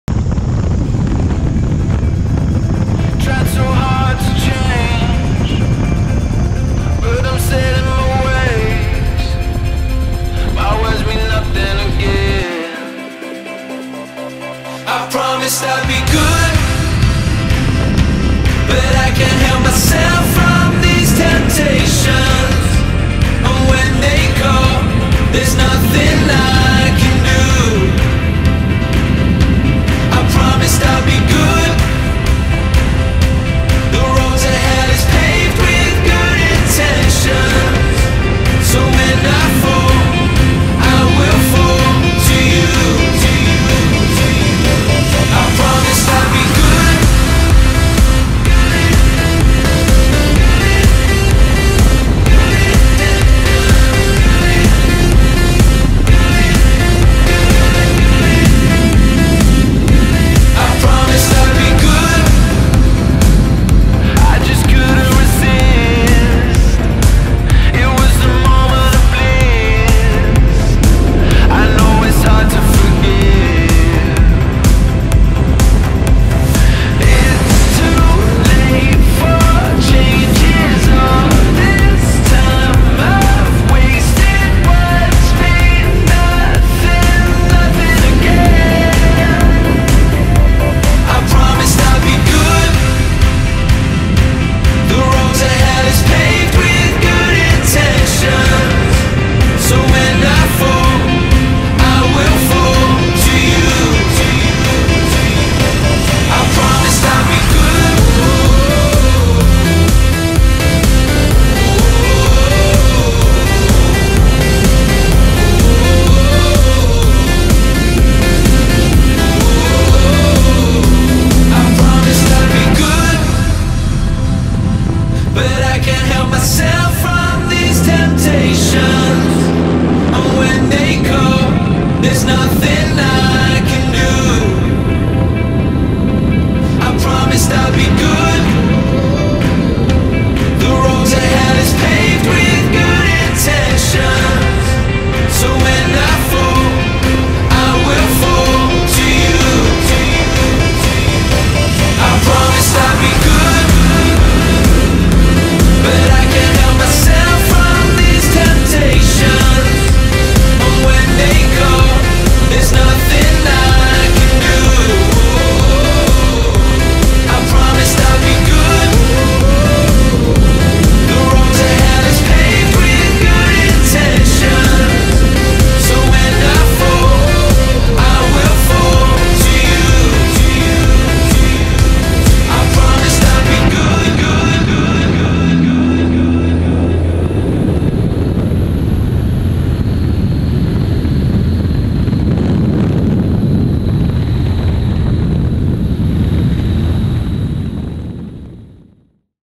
Akrofest 2019: Sportsman in a 2T-1a-2 Great Lakes Biplane
My last flight of Akrofest 2019 in Borrego Springs, California—home of the world's finest aerobatic box. This is the Sportsman Known sequence in a Great Lakes biplane. I had zero outs and the final score for this flight was 1083.5 out of 1290.0, or 83.99%